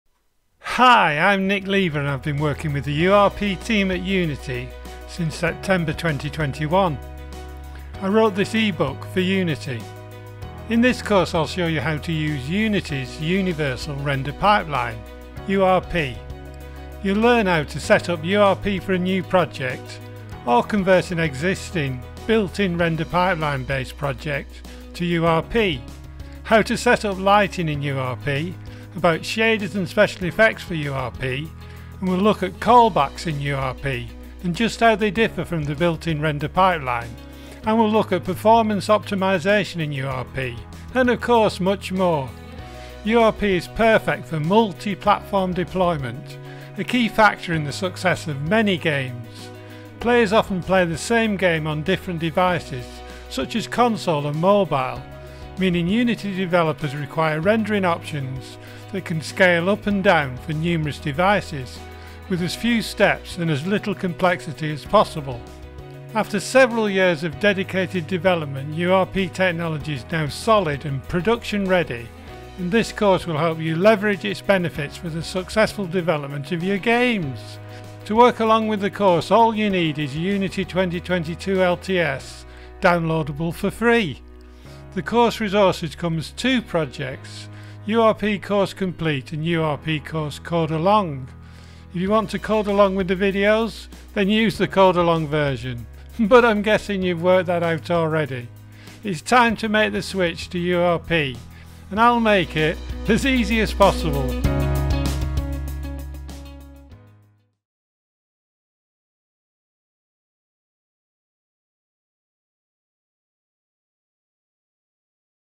The Complete Guide to Unity's Universal Render Pipeline (URP)
Promotional video for my Unity URP course available at a discount